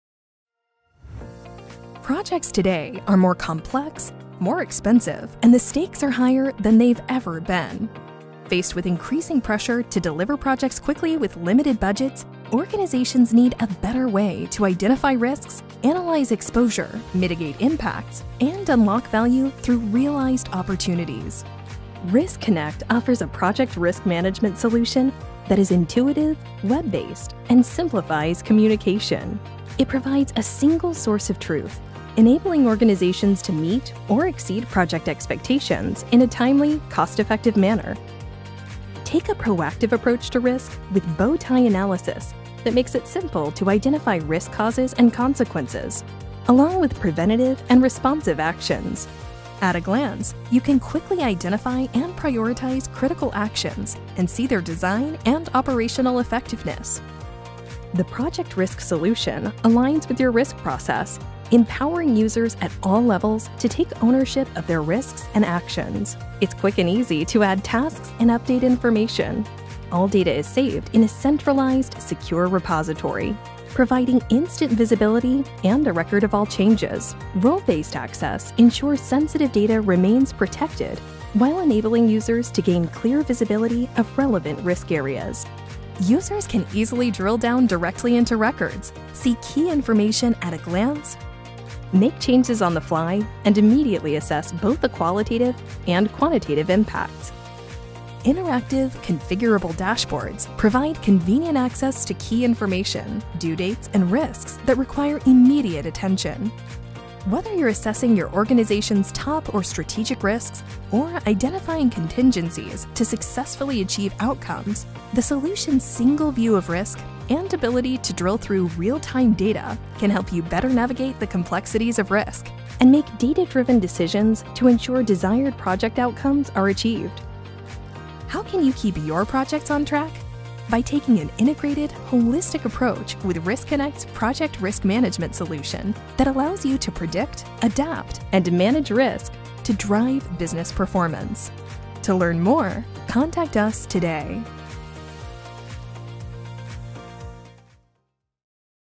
Riskonnect Active Risk Manager (Project Risk) | Solution Demo
In this video, we'll show you what Riskonnect's Active Risk Manager solution can do for your organization.